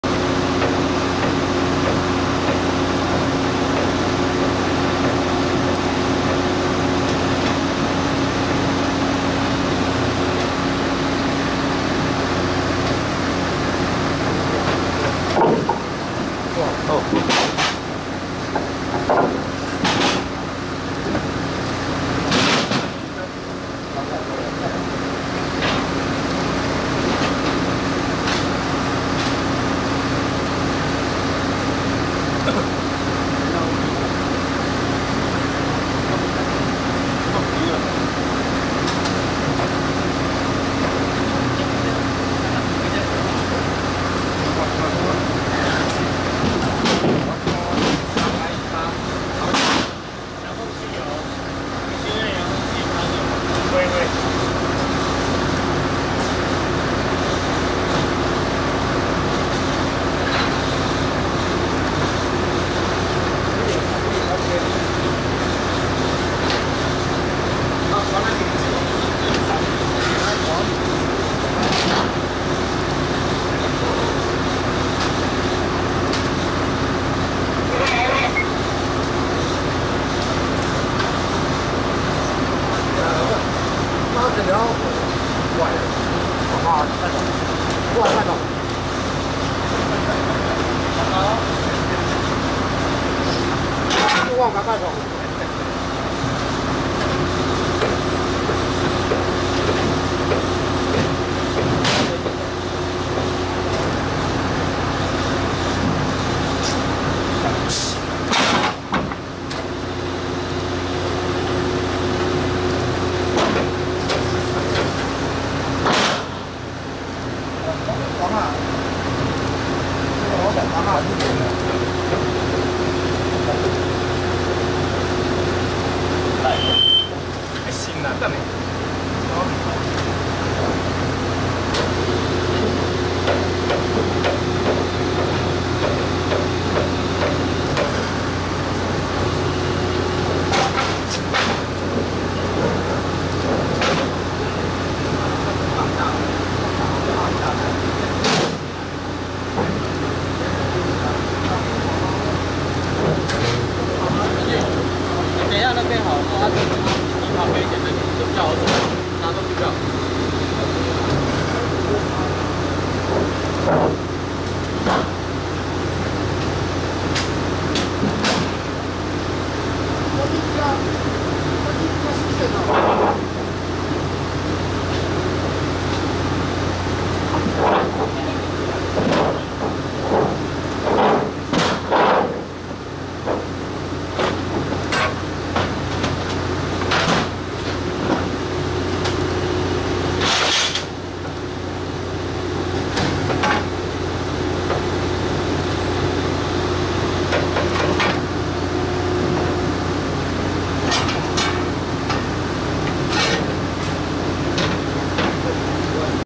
2015.5.13 台鐵 平溪線 幼坑橋 (魚腹橋) 更換鋼梁作業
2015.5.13 台鐵 平溪線 幼坑橋 (魚腹橋) 更換鋼梁作業